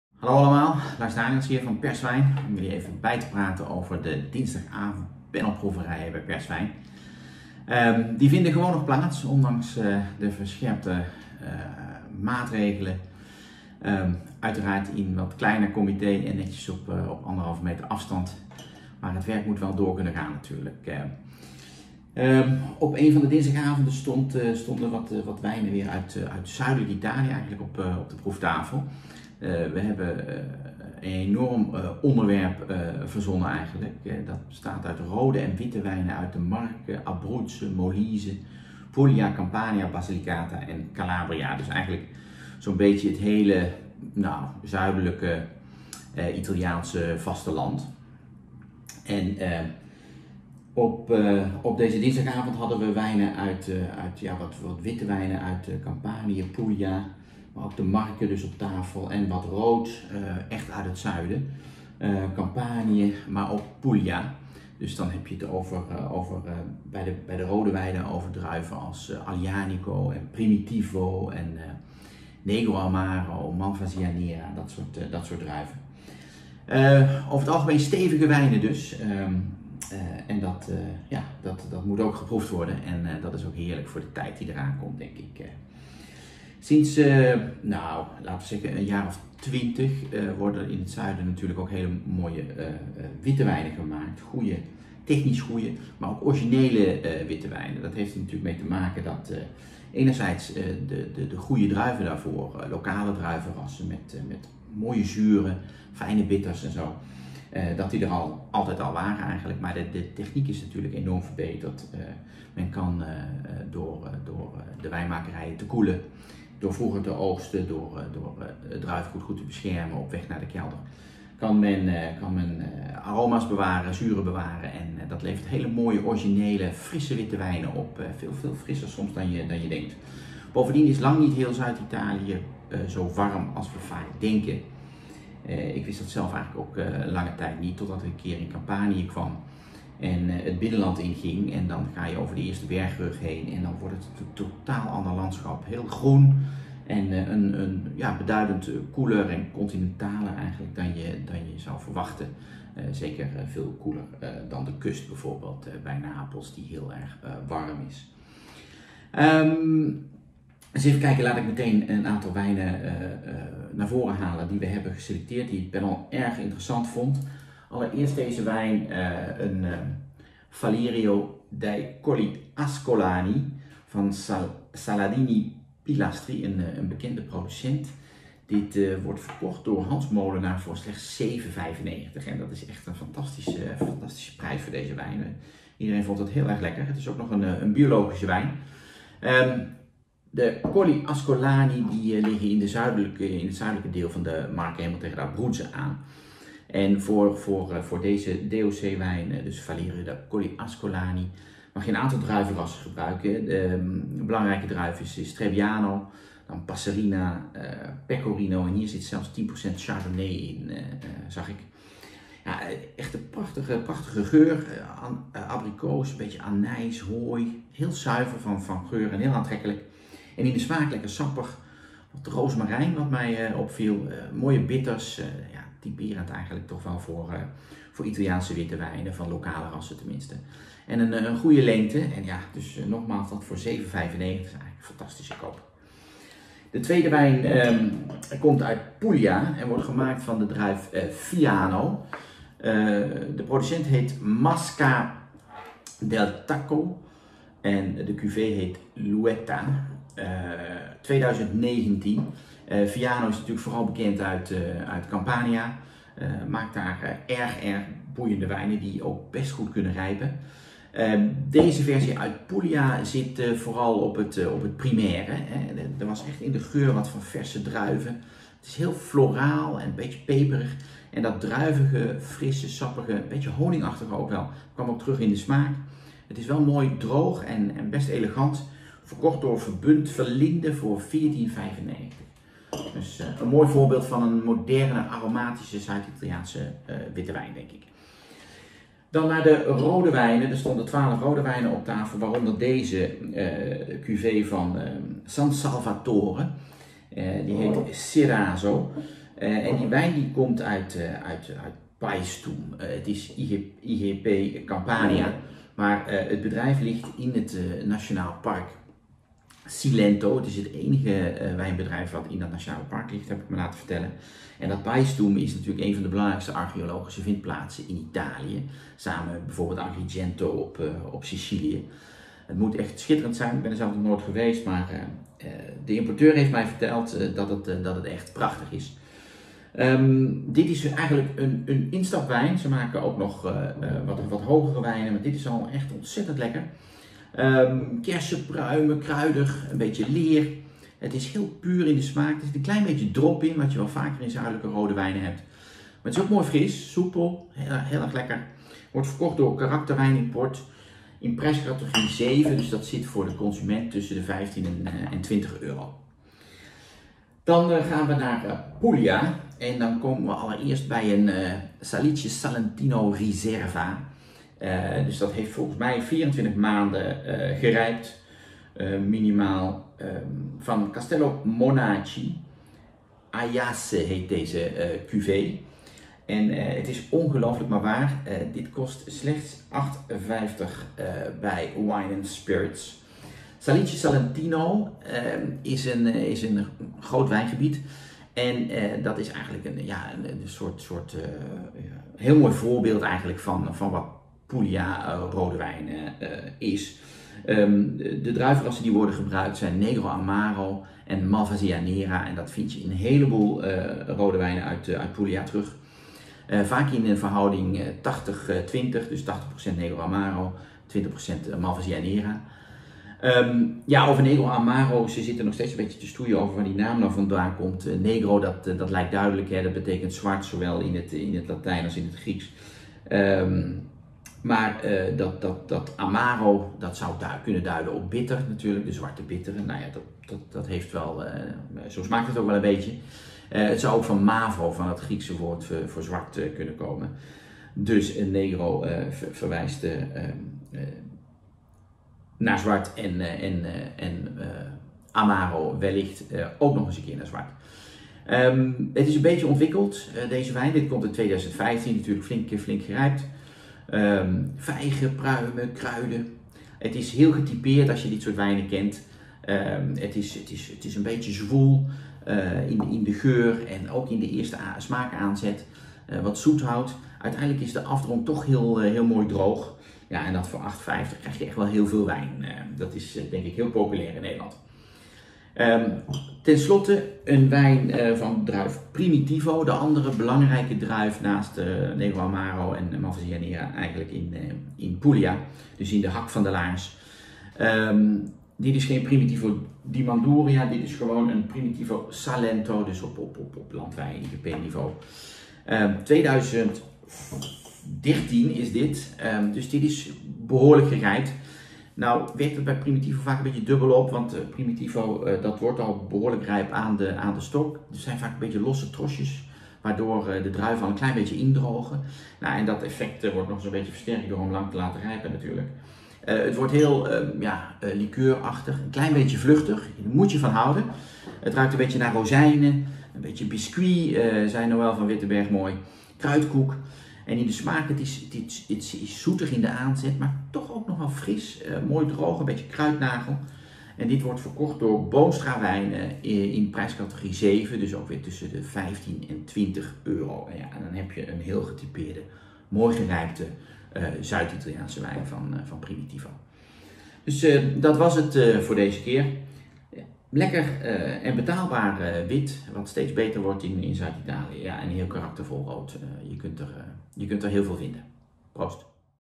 Aanbevolen wijnen: Rode en witte wijnen uit Zuid-Italië (Marche, Puglia, Campania)
Lars Daniëls MV licht als sneak preview op de rubriek ‘Geproefd en Beoordeeld’ in het magazine, enkele wijnen uit die opmerkelijk zijn bevonden tijdens het proeven en dus van harte worden aanbevolen. De volledige proefnotities kunt u vinden in een van de aanstaande edities van Perswijn.

Dit zijn de uitgelichte wijnen van deze week:
Saladini Pilastri, Falerio 2019, Hans Moolenaar Wijnkoperij (€ 7,95)
Masca del Tacco, L'Uetta Fiano Puglia 2019, Verbunt Verlinden (€ 14,50)
San Salvatore, Ceraso Aglianico Paestum 2018, Karakter Wijnimport / Wijnkoperij Europa (Prijscategorie 7: €15,- tot €20,-)
Castello Monaci, Salice Salentino Riserva Aiace 2015, Wine & Spirits (€ 8,50)
Miali, Mater Primitivo 2013, Boonstra Wijnen (Prijscategorie 7: €15,- tot €20,-)

Voor elke editie van PERSWIJN Magazine hebben we 500 wijnen geproefd en beoordeeld.